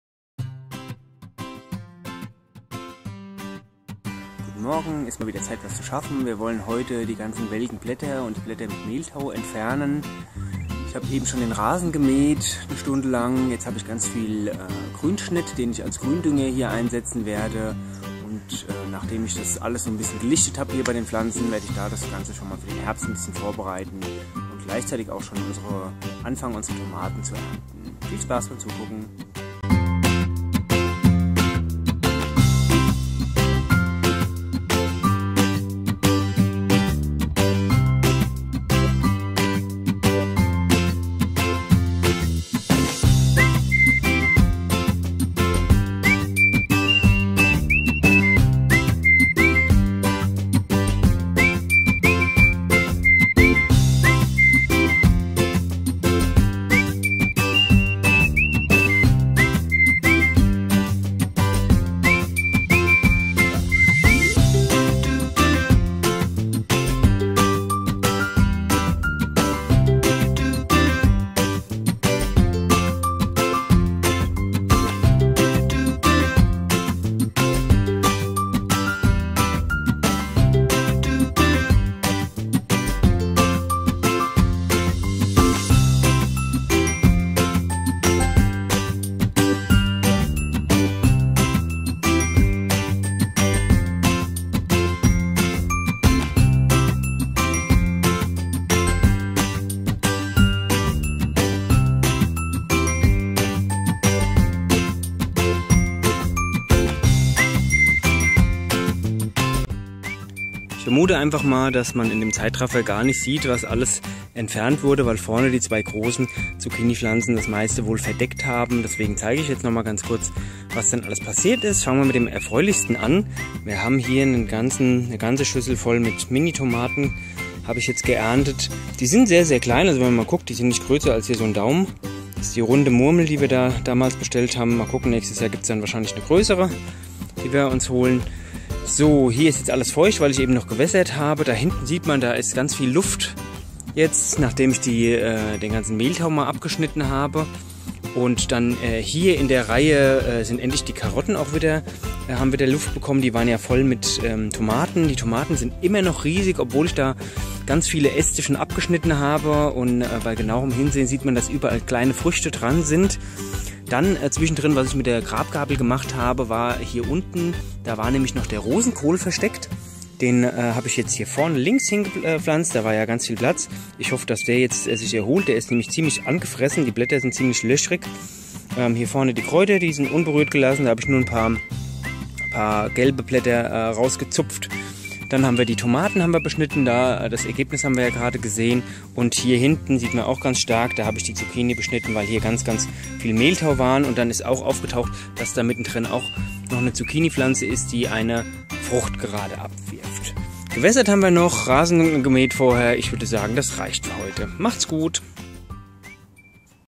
Borstens Beet #35 - Beet lichten, Tomaten ernten
Heute lichten wir das Beet etwas da die Zucchini-Blätter etwas Mehltau bekommen haben und etwas Unkraut sich auch breit gemacht hat. Daneben ernte ich ein paar der kleinen Tomaten "Runde Murmel".